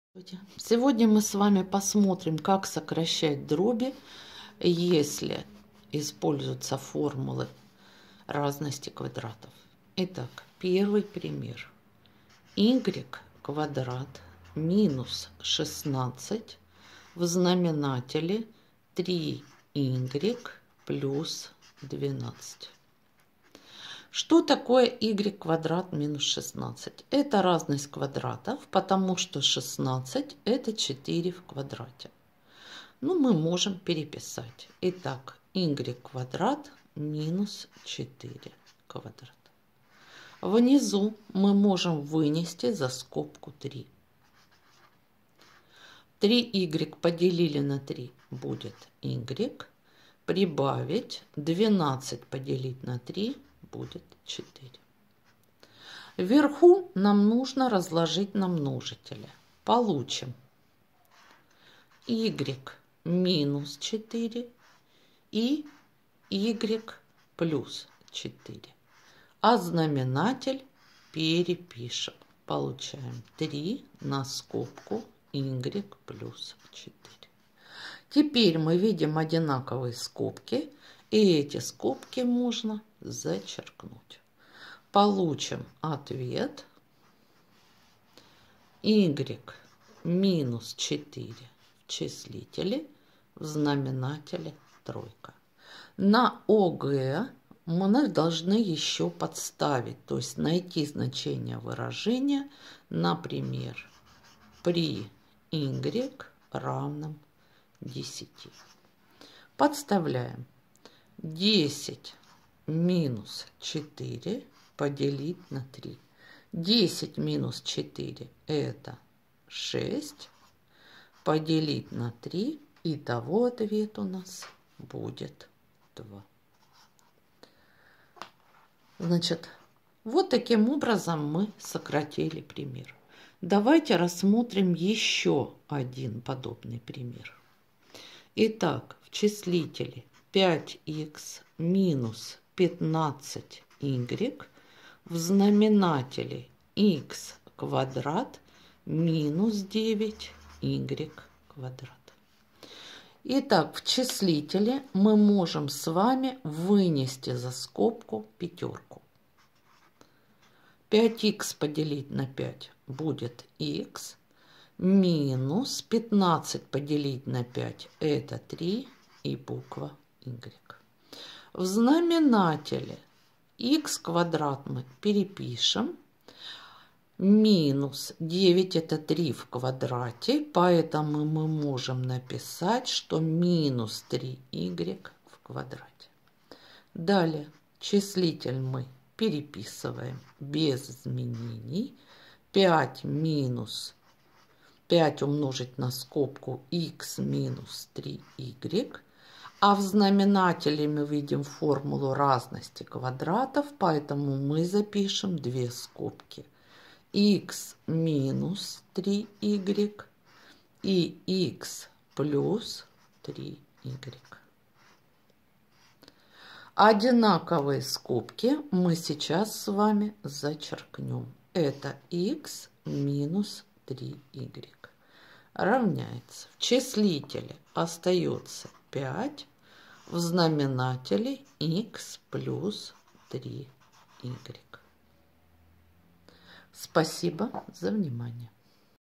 Огэ,7 и 8 кл.Сокращение рациональных дробей при помощи формулы разности квадратов
Сокращение рациональных дробей при помощи формулы разности квадратов